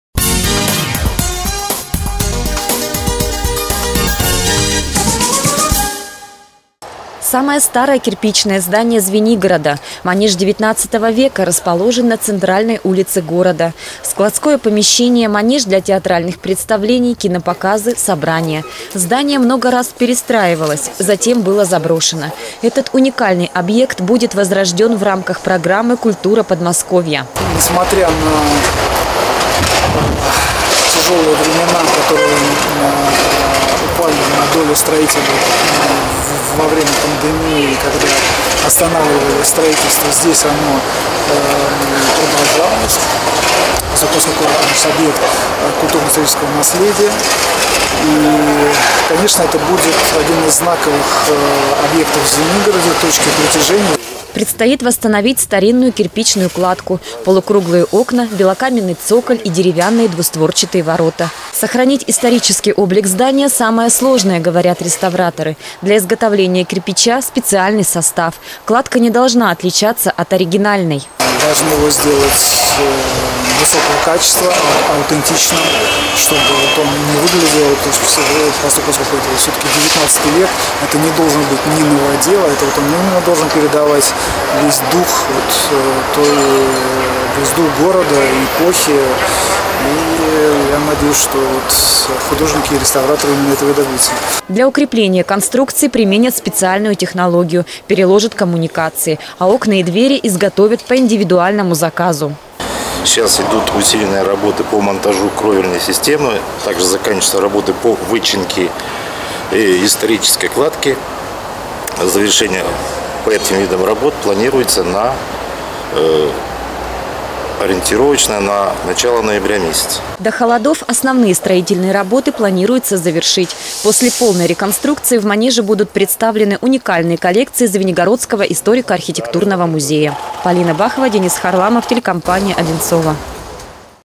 Возрождение звенигородского манежа
Самое старое кирпичное здание Звенигорода. Манеж 19 века. Уникальное здание много раз перестраивалось, затем было заброшено. Теперь его восстанавливают по программе «Культура Подмосковья».